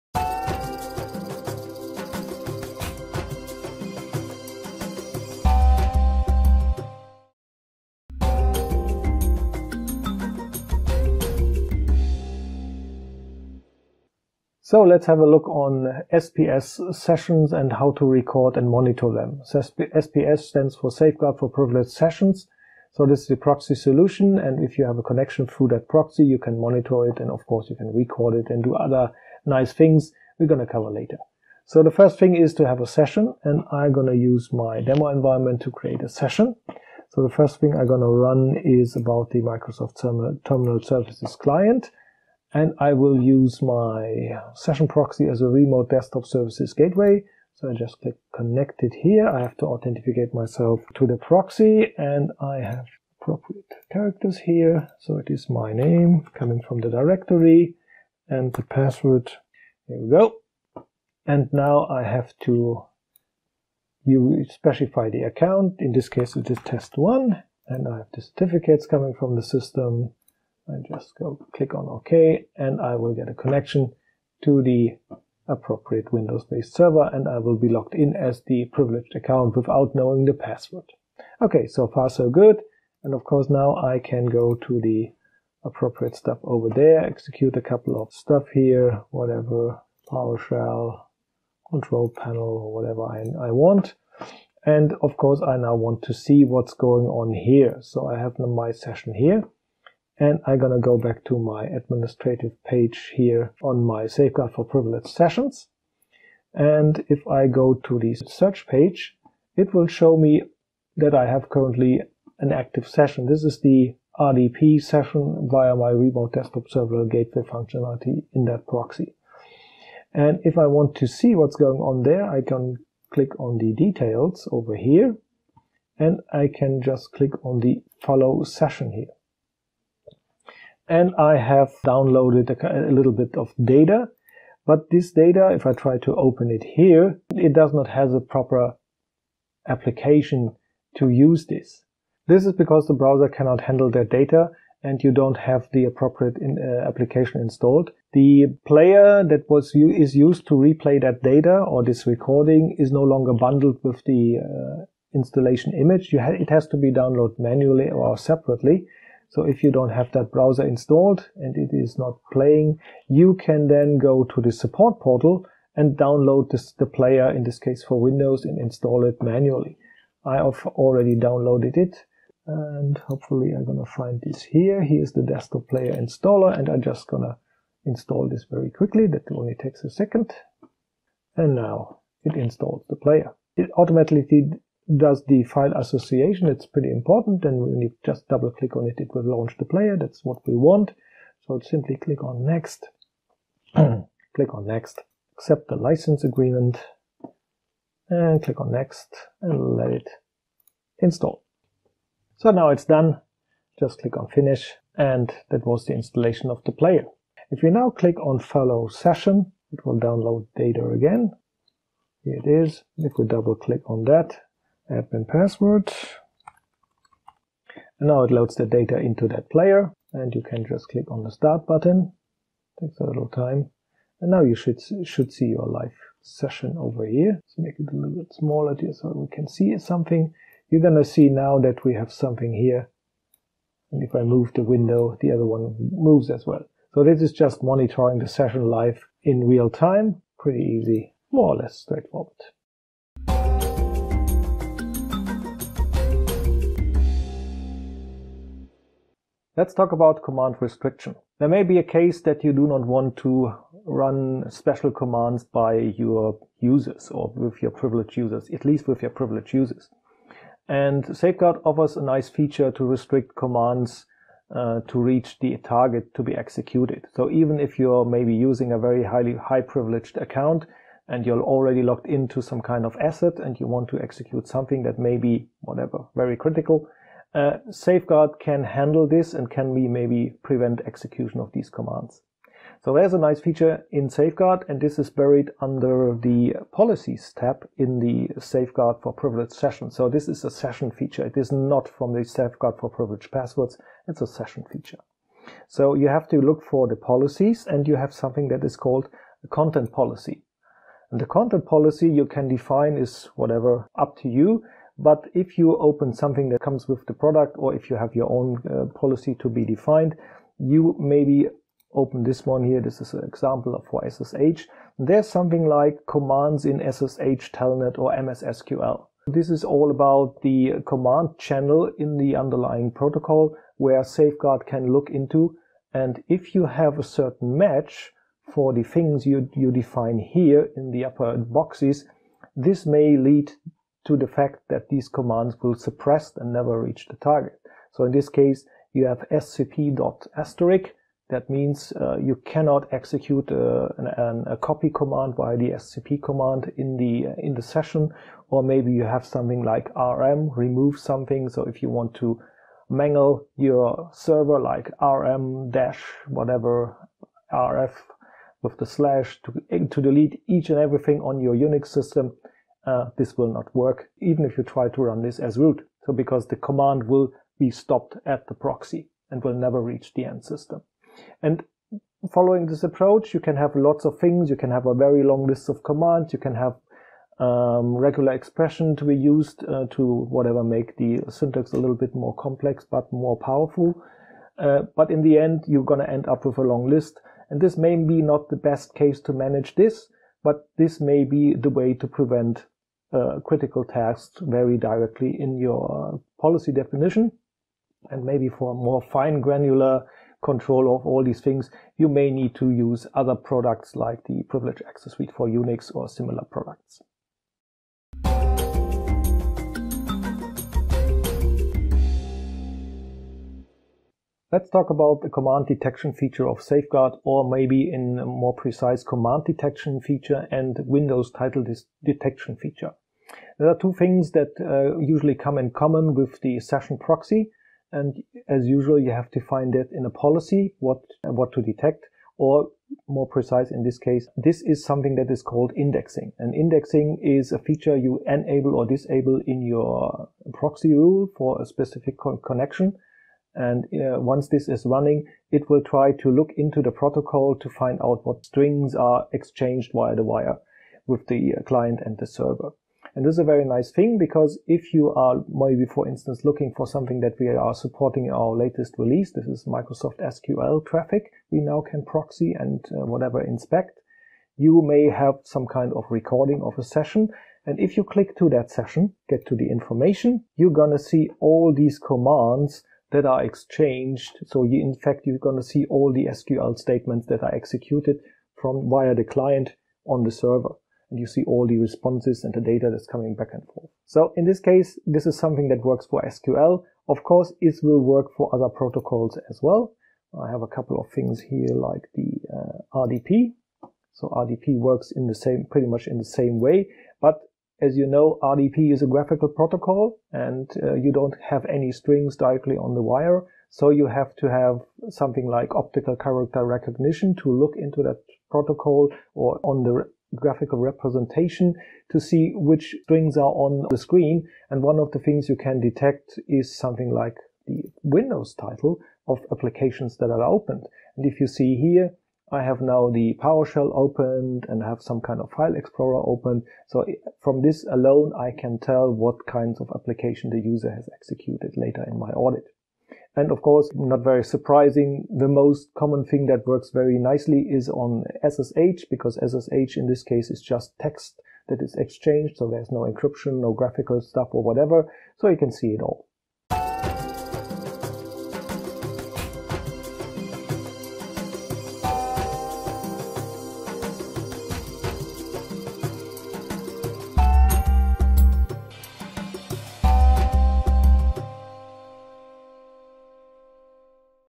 One Identity Safeguard | Technical Overview #14 | Session monitoring and Cmd restriction/detection
Training Video Series: One Identity Safeguard - Technical Overview #14
Part 3: Technical Details
Session monitoring and Command restriction/detection
- Session Monitoring
- Command restriction
- Command detection